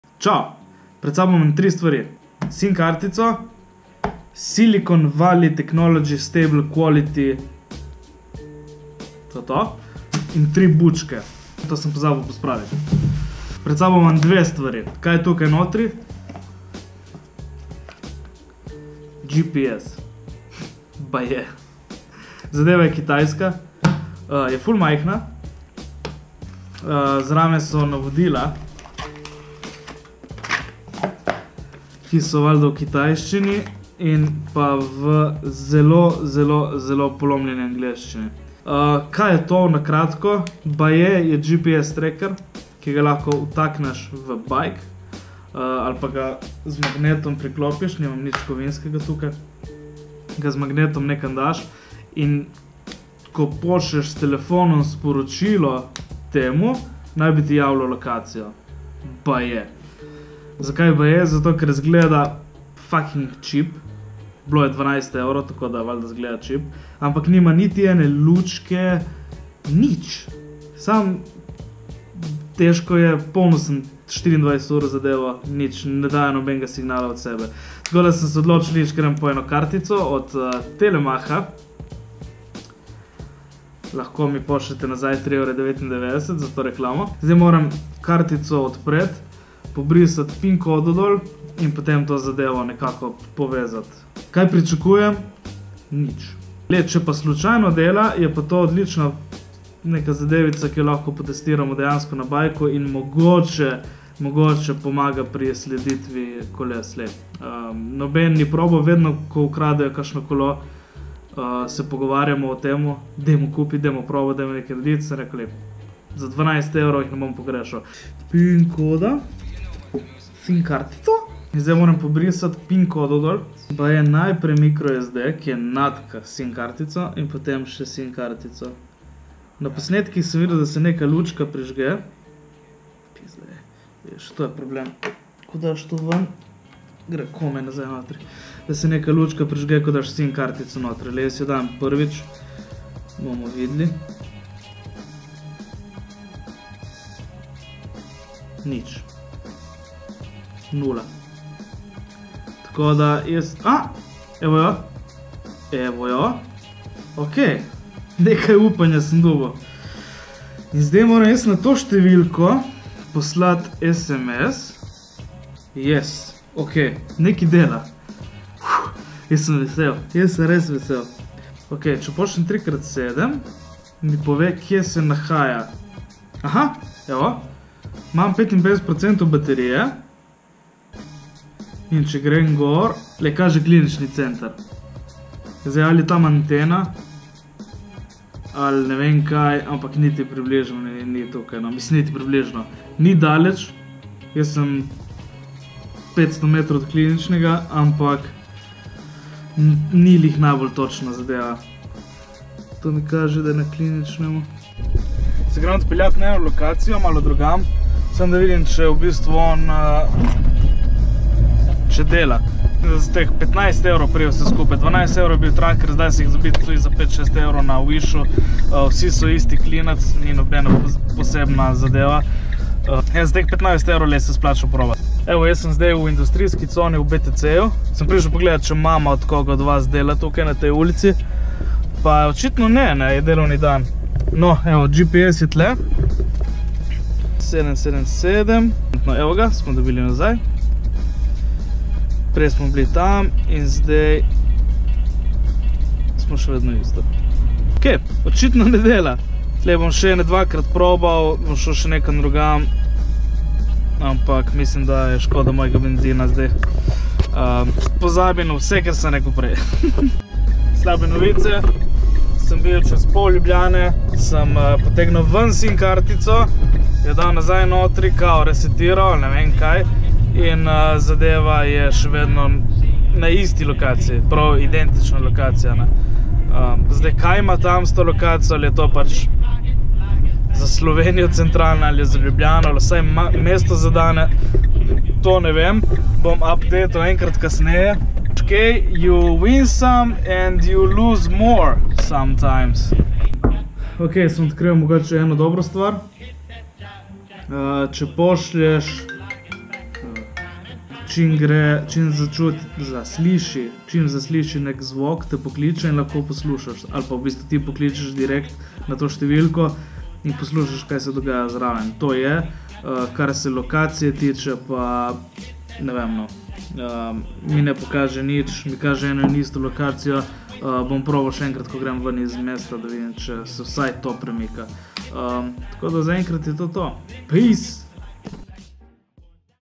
GPS Tracker za 12 evričev?
Kaj je črno, trdi in iz kitajske?
Ne ni moj dildo ampak 'GPS tracker'.

Počekiraj, če te zanima kako in če dela. ;)